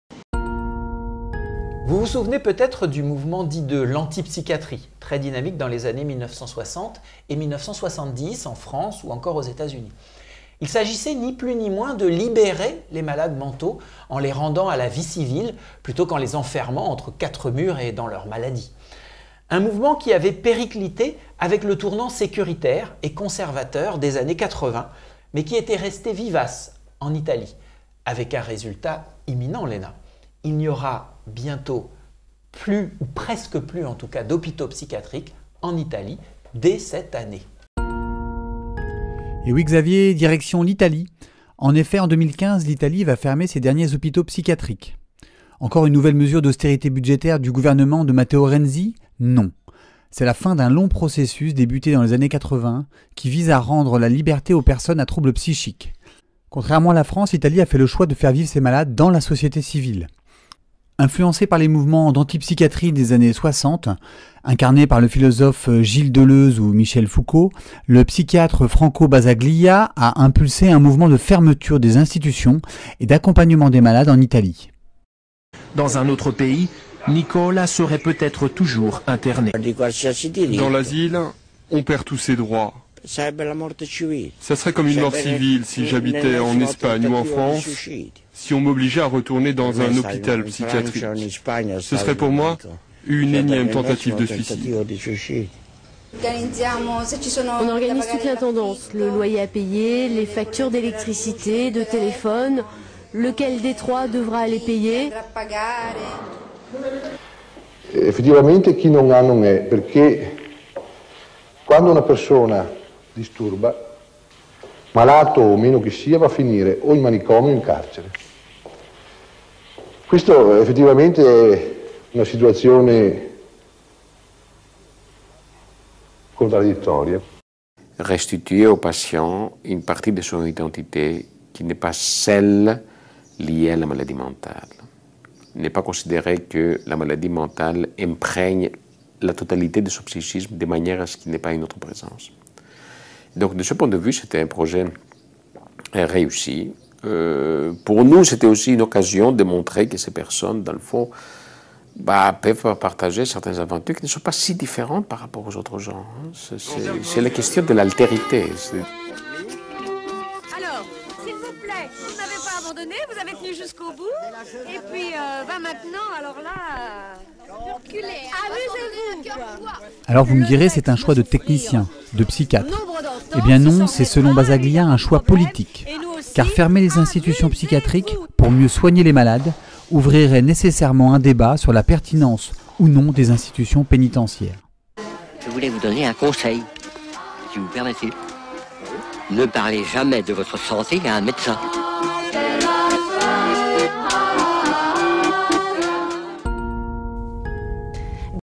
La psychiatrie en Italie - Reportage AlterJT 21/02/2015 - 17
En 2015, l'Italie va fermer la quasi-totalité derniers hôpitaux psychiatriques...

Retrouvez l'AlterJT sur:
Pour nous soutenir :